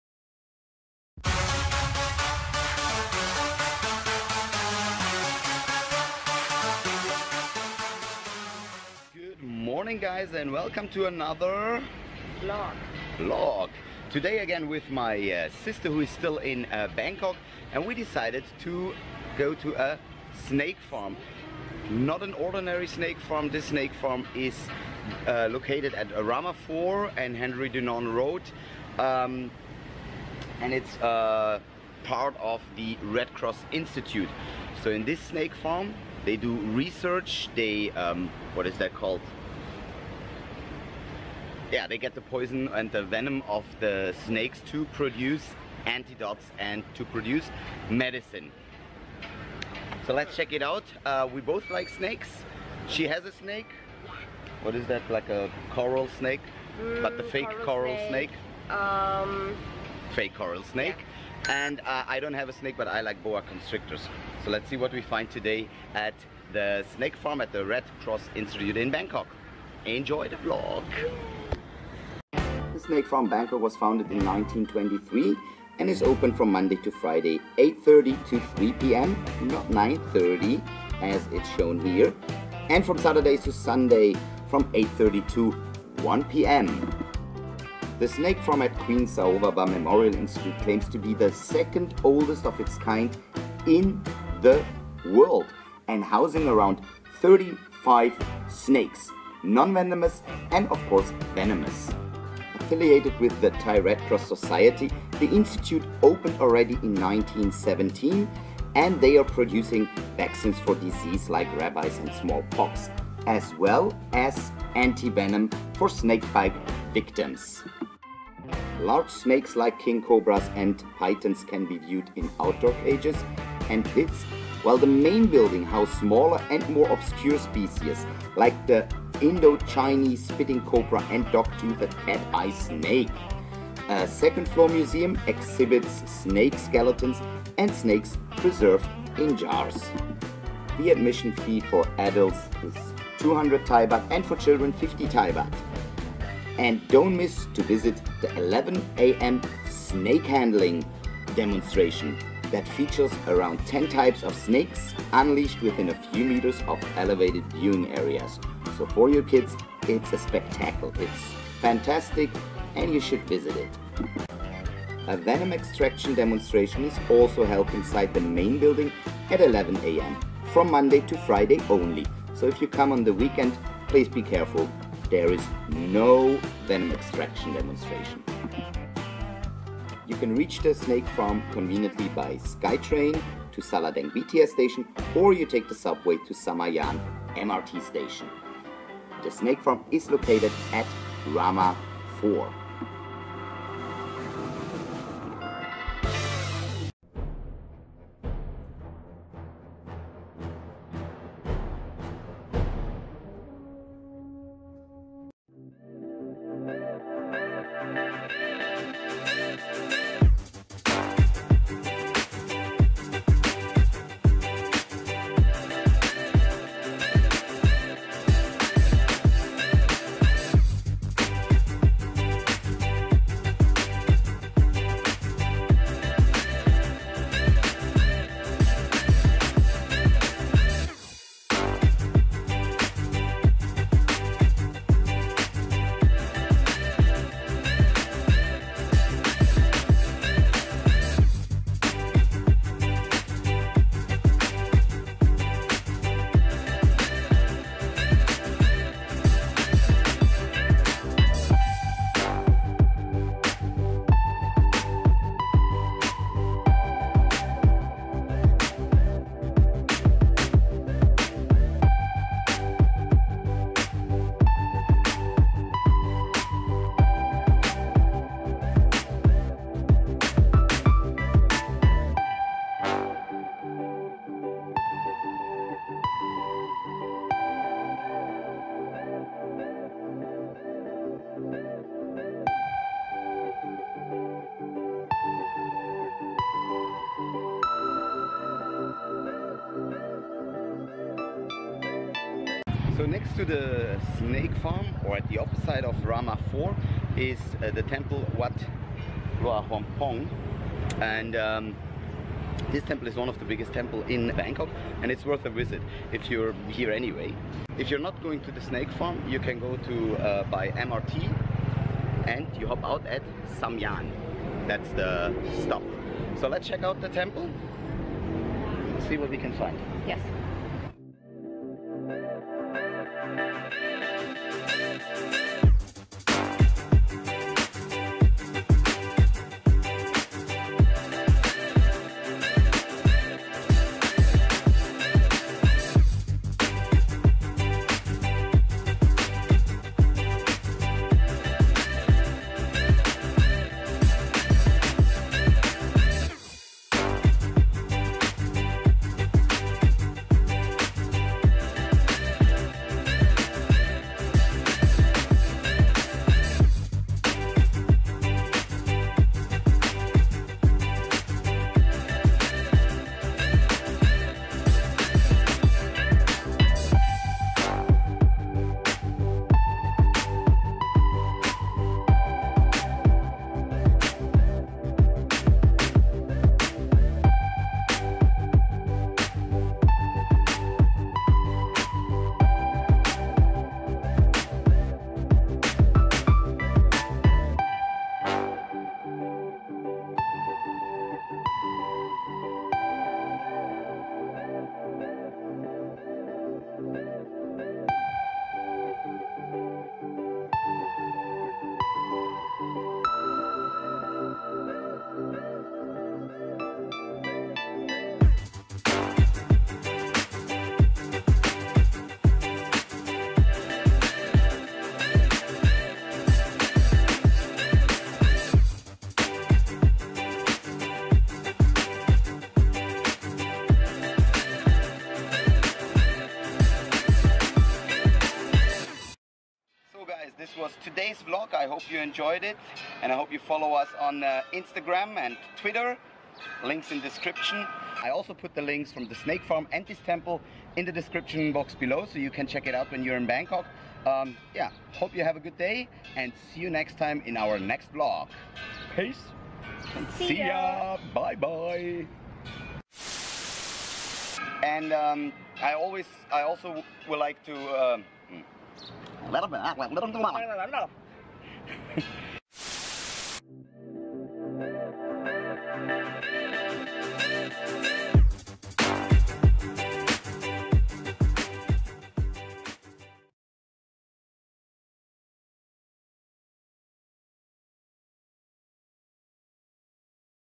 SNAKES IN BANGKOK - Best Snake Farm in Thailand - Largest Temple in Bangkok
Traveling to Thailand means you will sooner or later come to Bangkok. The snake farm has around 35 species (venomous and non-venomous) and produce antidotes for snake bite victims. It is not an ordinary snake farm and worth a visit. The snakes are treated well and it serves an educational purpose. You will see a lot of snakes, from King Cobra to Boa Constrictor, Pythons, Vipers, water snakes, coral snake and more.

Located right next to the Snake farm, opposite of Rama IV is Wat Hua Lamphong, one of the largest temples in Thailand. While you there visit this temple too. Enjoy the video.

The vlog was filmed with a Canon G7X Mark II